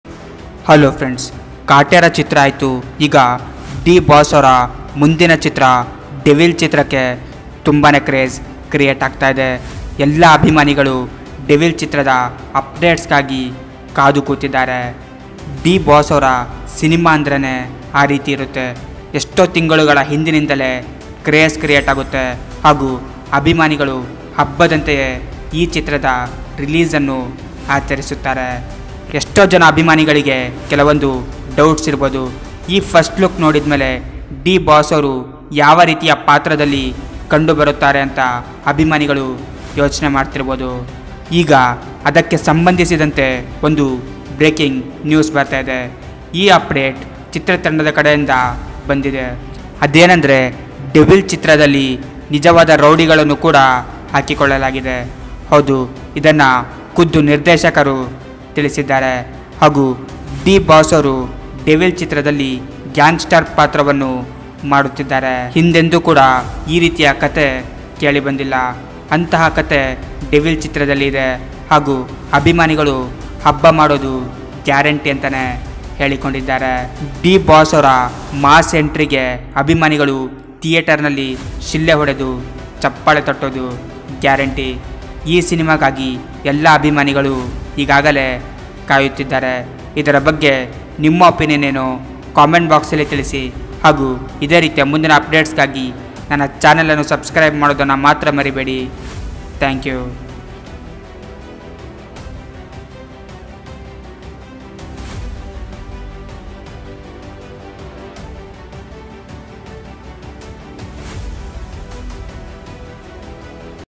ಅಬ್ಬಾ ! ಡೆವಿಲ್ ಚಿತ್ರದಲ್ಲಿ ನಿಜವಾದ ರೌಡಿಗಳು ! Gangster ಪಾತ್ರದಲ್ಲಿ D Boss ! ಇದು ನಿಜಕ್ಕೂ ಶಾಕಿಂಗ್ ನೋಡಿ 🔥
This video gives information that real rowdies are acting in devil movie